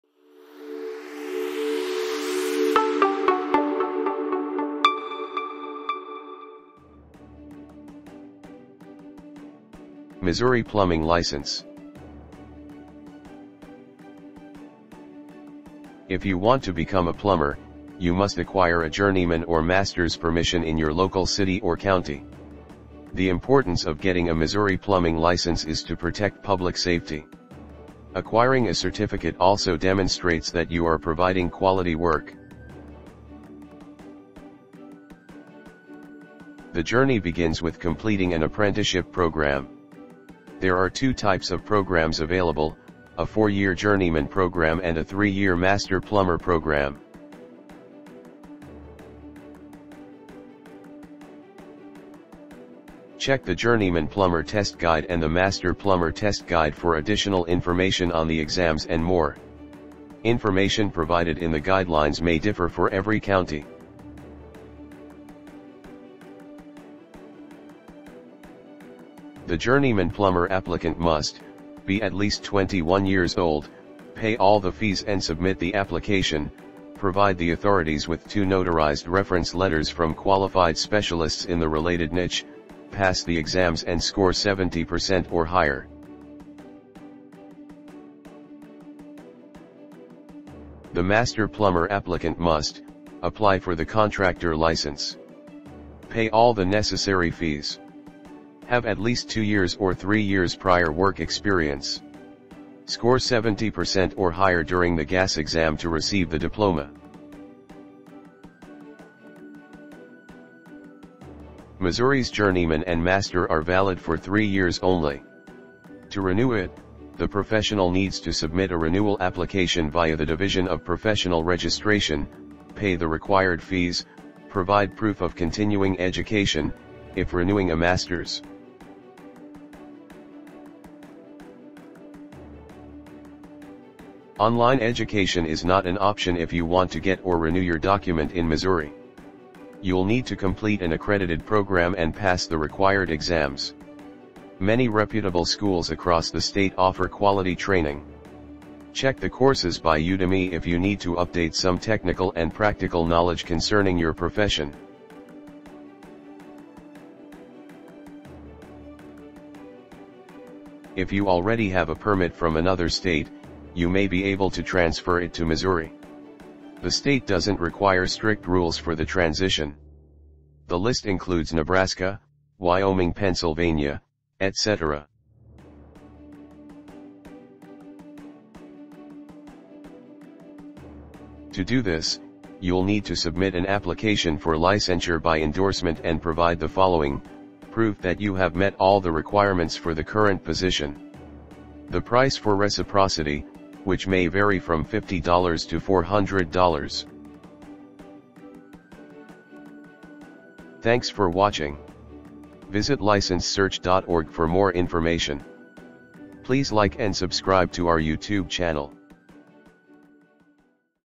Missouri Plumbing License - What You need to get started
If you want to get a Missouri Plumbing License, you should know there is no general license. Here you can find how to apply for your license.